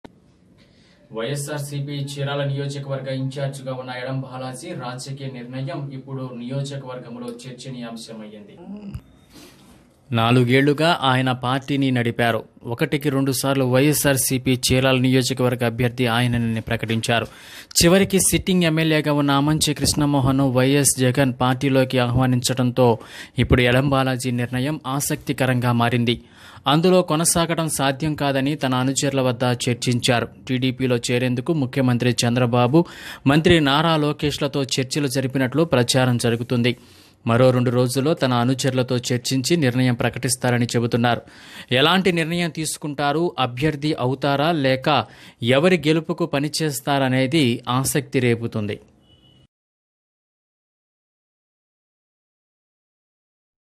Chirala:-వైసిపి ఇన్ఛార్జి యడం బాలాజీ టిడిపిలో చేరుతున్నట్లు ప్రచారం|| Dn5
వైఎస్ఆర్సిపి చీరాల నియోజకవర్గ ఇన్ఛార్జిగా ఉన్న యడం బాలాజీ రాజకీయ నిర్ణయం ఇప్పుడు నియోజకవర్గంలో చర్చనీయాంశమైంది.
నాలుగేళ్లుగా ఆయన పార్టీని నడిపారు. ఒకటికి రెండు సార్లు వైఎస్ఆర్సిపి చీరాల నియోజకవర్గ అభ్యర్ధి ఆయనేనని ప్రకటించారు. చివరికి సిట్టింగ్ ఎంఎల్ఎగా ఉన్న ఆమంచి కృష్ణమోహన్ను వైఎస్ జగన్ పార్టీలోకి ఆహ్వానించడంతో ఇప్పుడు యడం బాలాజీ నిర్ణయం ఆసక్తి కరంగా మారింది. అందులో కొనసాగడం సాధ్యకాదని తన అనుచరుల వద్ద చర్చించారు. టిడిపిలో చేరేందుకు ముఖ్యమంత్రి చంద్రబాబు, మంత్రి నారా లోకేష్లతో చర్చలు జరిపినట్లు ప్రచారం జరుగుతుంది. మరో రెండు రోజుల్లో తన అనుచరులతో చర్చించి నిర్ణయం ప్రకటిస్తారని చెబుతున్నారు. ఎలాంటి నిర్ణయం తీసుకుంటారు?  అభ్యర్ధి అవుతారా?  లేక ఎవరి గెలుపుకు పనిచేస్తారనేది ఆసక్తి రేపుతుంది.